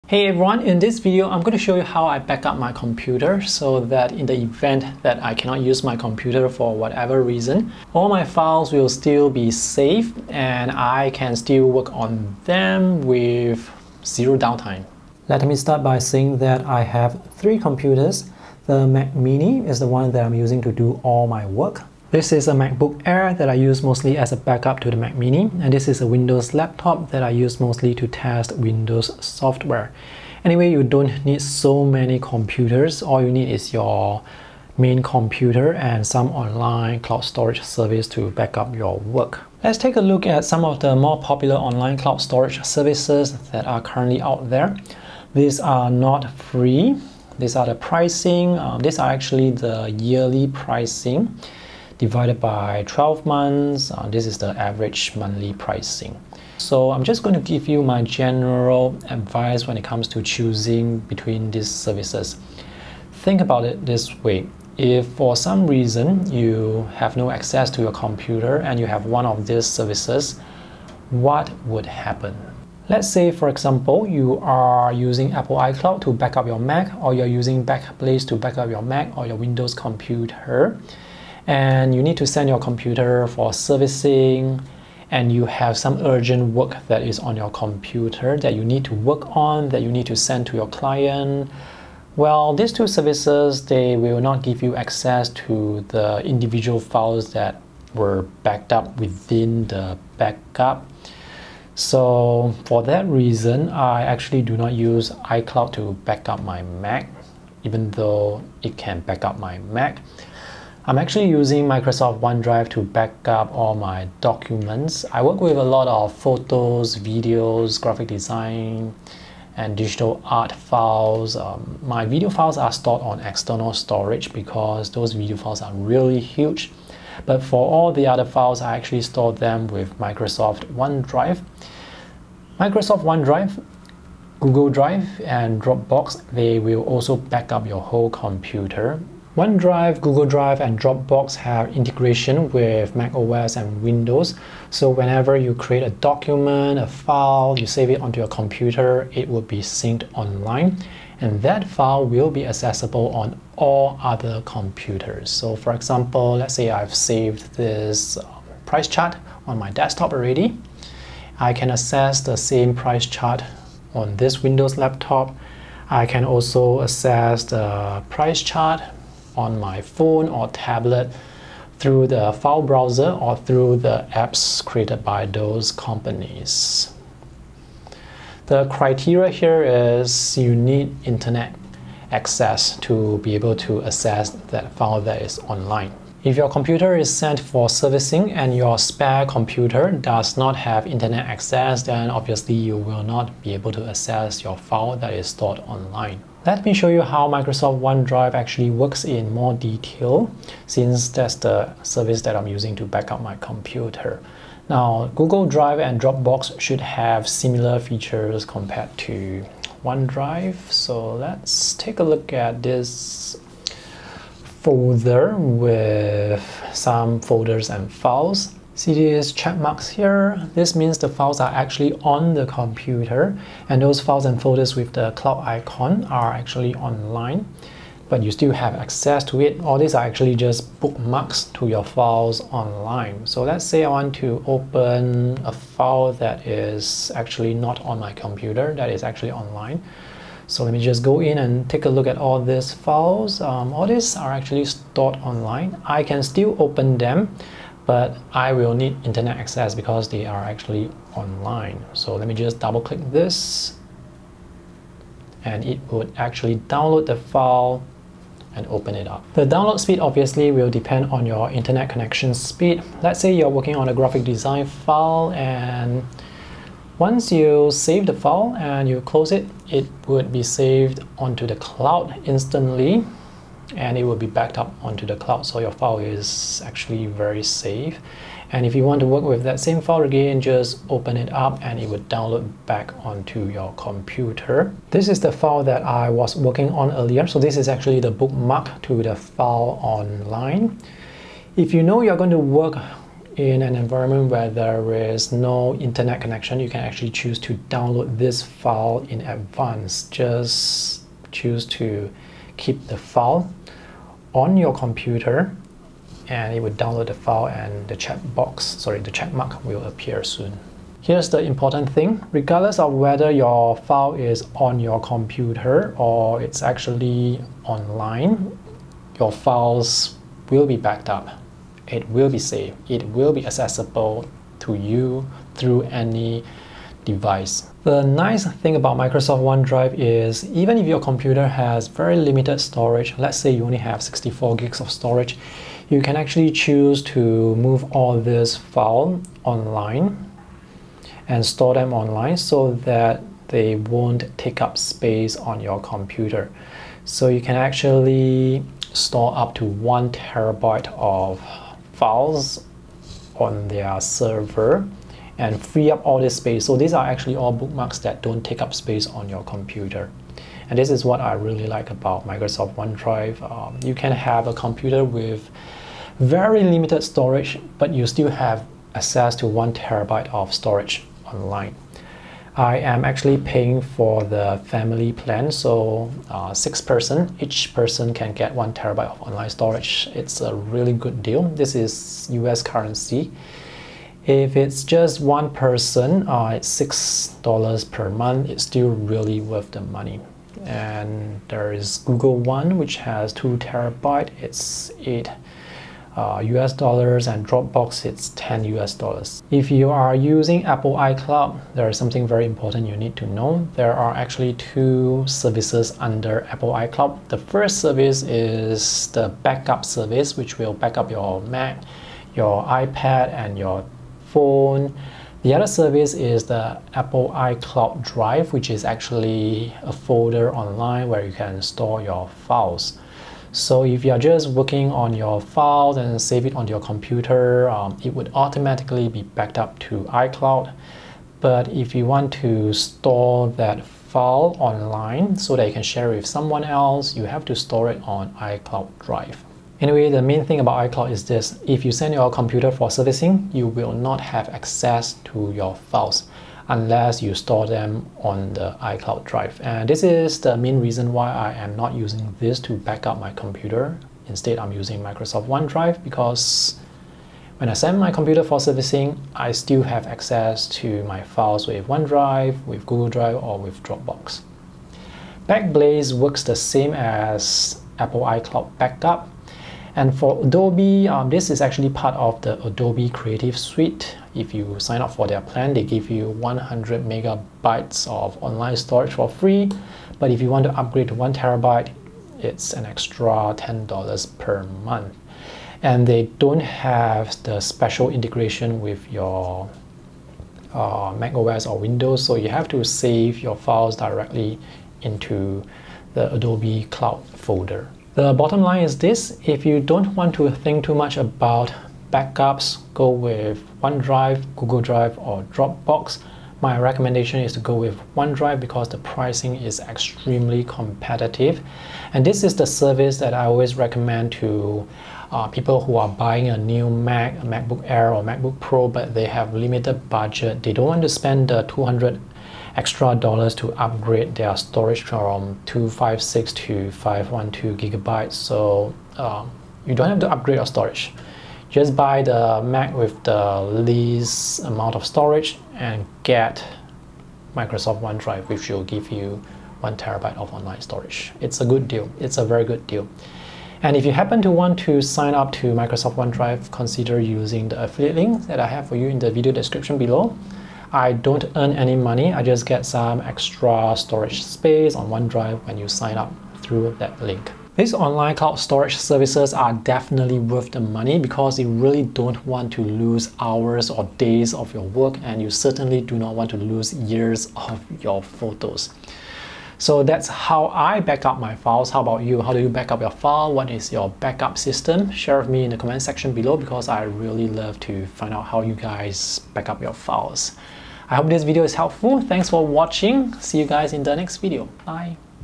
How I Backup My Computer: iCloud vs OneDrive vs Others
I use Microsoft OneDrive to backup my computer. iCloud to backup my iPad and iPhone.